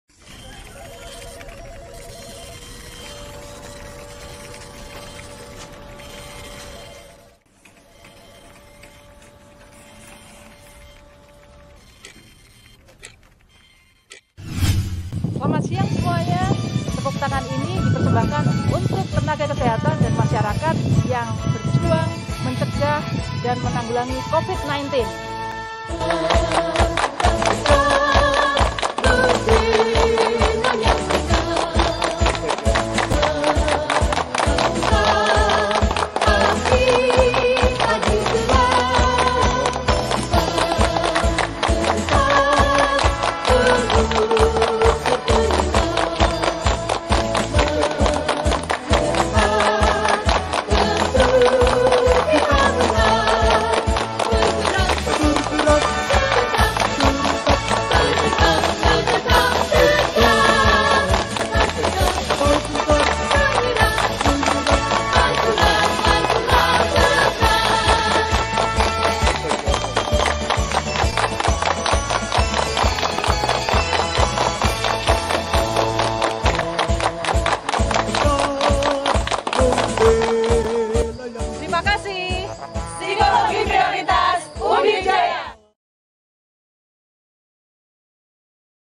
Fakultas Psikologi Universitas Diponegoro: Hari Kesehatan Nasional Ke-56
Memperingati hari kesehatan nasional ke-56, kami mengajak seluruh Individu/Kelompok/Institusi yang peduli terhadap pencegahan dan penanganan COVID-19 di Indonesia
Sebagai penghargaan kepada tenaga kesehatan yang telah berjuang dan masyarakat yang telah melaksanakan protokol kesehatan untuk mencegah COVID-19 melalui tepuk tangan secara bersama selama 56 detik